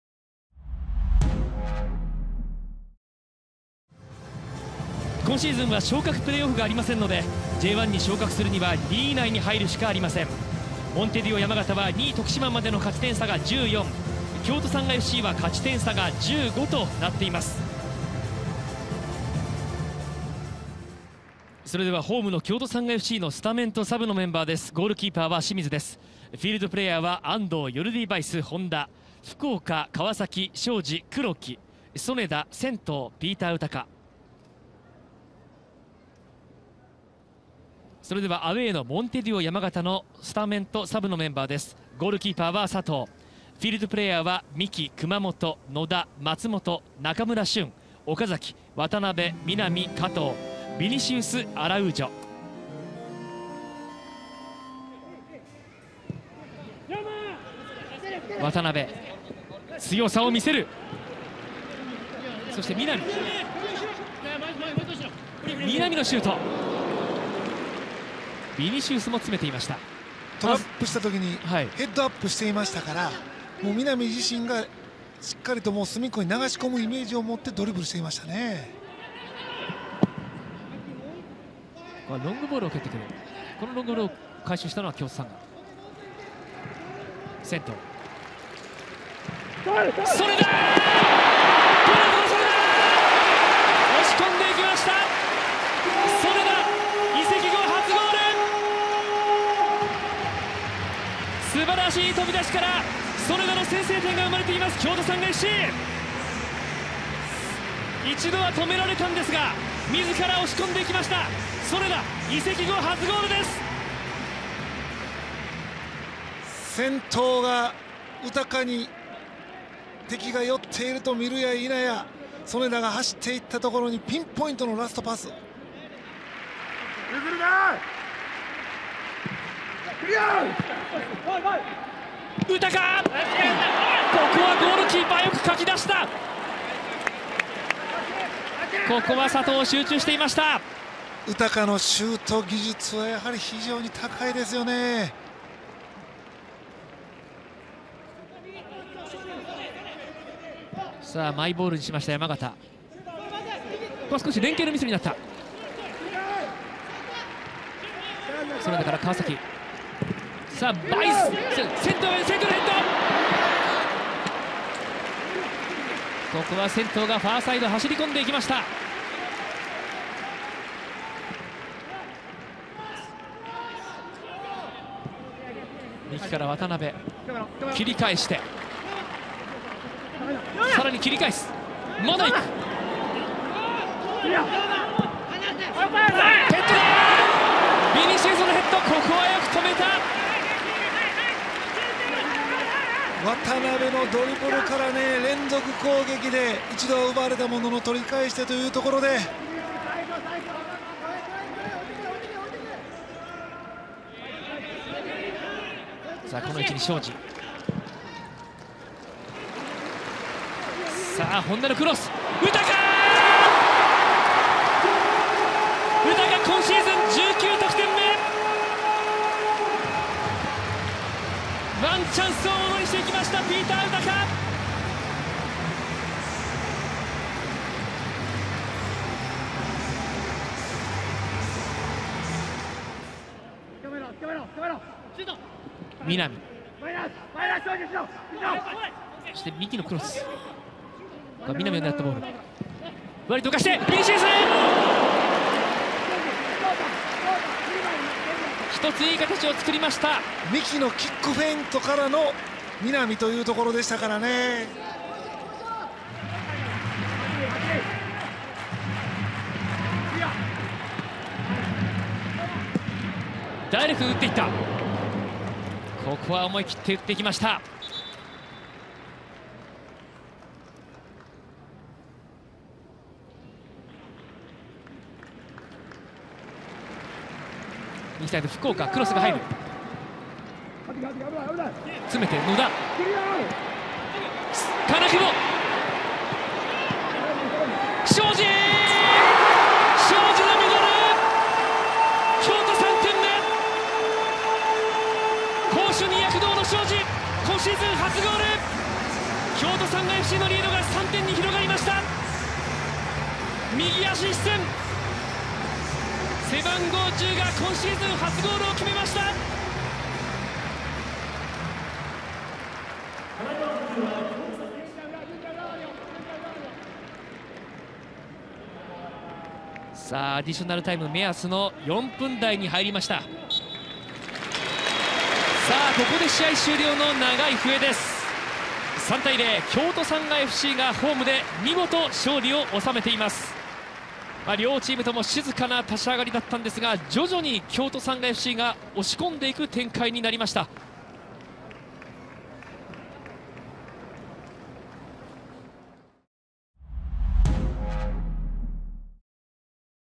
【ハイライト】2020明治安田生命J2リーグ第30節vs.山形＠サンガS
2020年11月1日（日）14:03キックオフ
京都サンガF.C. 3-0（前半1-0）モンテディオ山形
[得点者]曽根田穣、ピーターウタカ、庄司悦大
[会場]サンガスタジアム by KYOCERA

【NEXT HOME GAME】
2020年11月4日（水）19:00キックオフ
京都サンガF.C. vs. ファジアーノ岡山